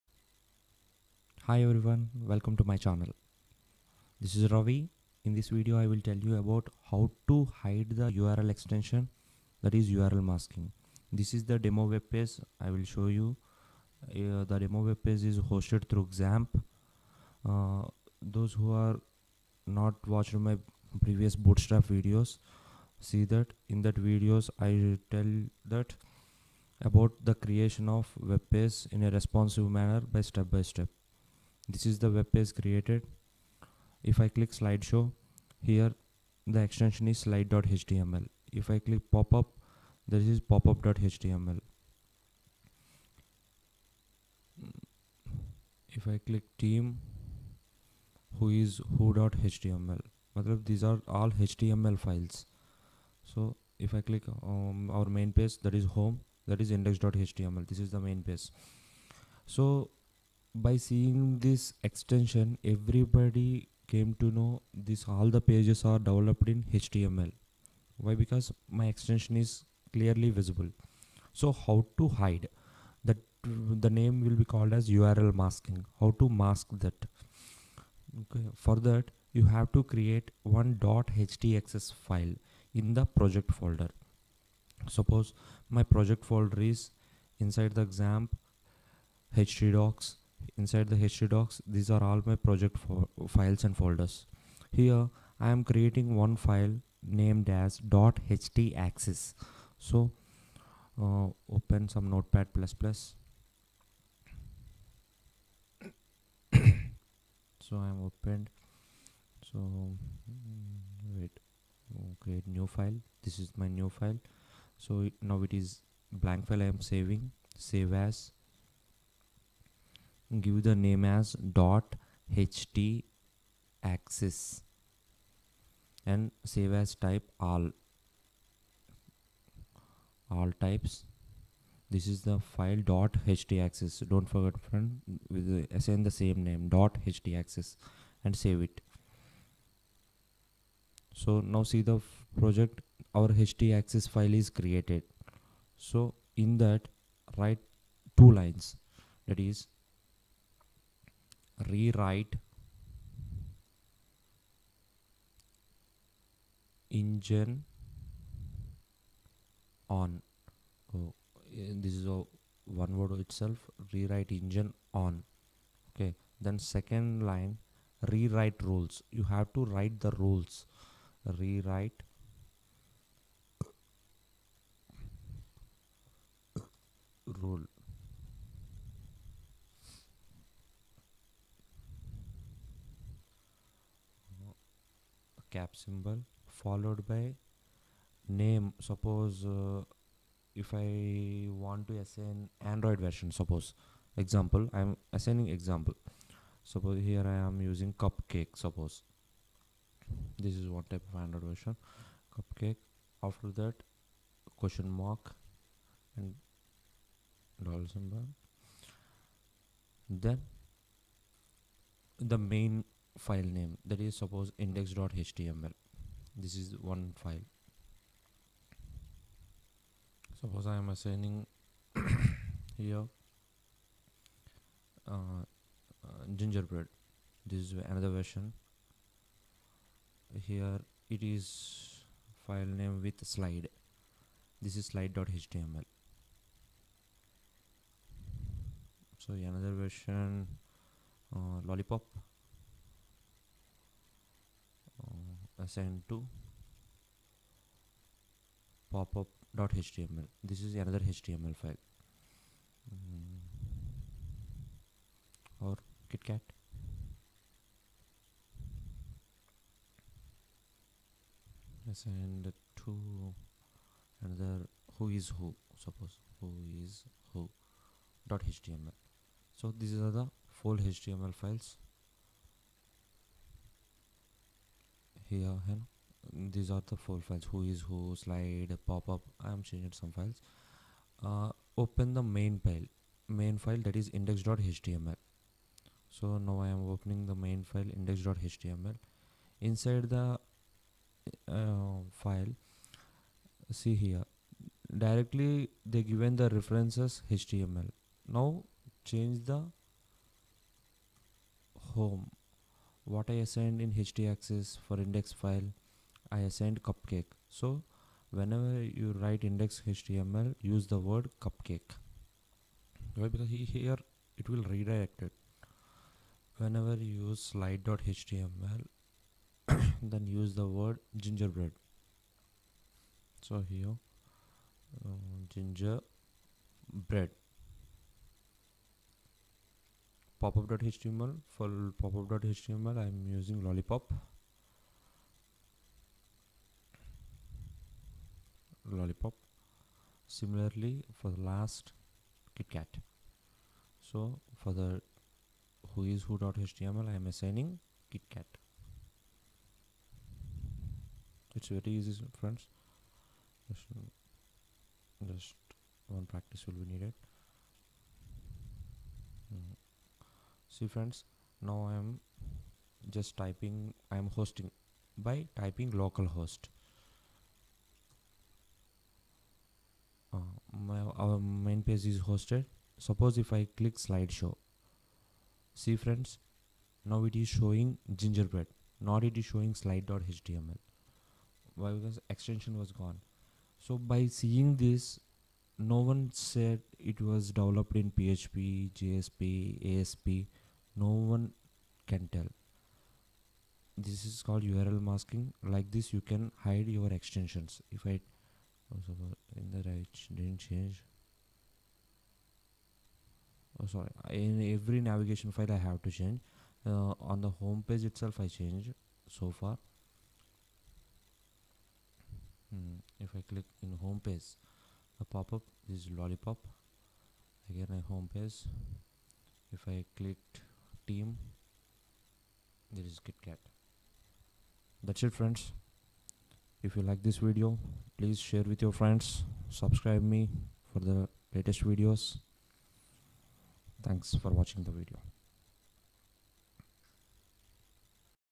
Hide URL Extension or URL Masking
Disable the Domain or URL extension by .htaccess file